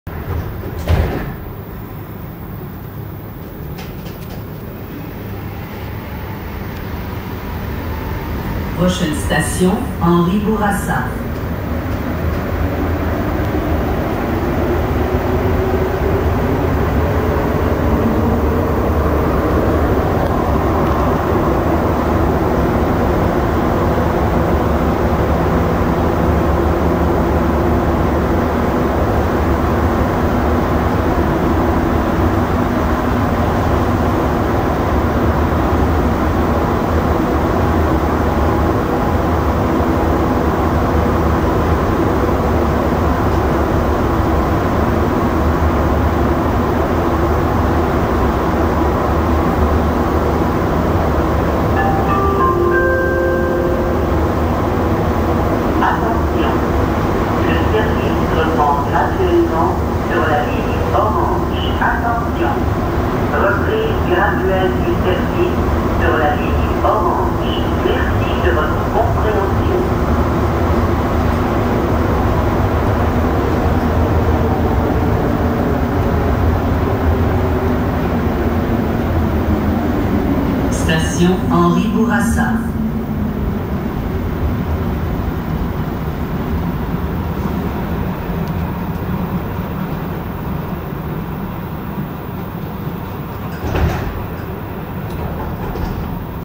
🚅 A short metro ride between Cartier Metro Station and Henri-Bourassa Station on the Orange Line 🟠
Here is a short metro ride between Cartier Metro Station in Laval and Henri-Bourassa Metro Station in Montreal on the Orange Line 🟠

The metro route takes us directly underneath the Rivière des Prairies (River of the Prairies) between Island of Laval and the Island of Montreal.

Cartier Metro Station is located in the Laval-des-Rapides neighborhood of Laval while Henri-Bourassa Station is located in the Ahuntsic neighborhood of Montreal.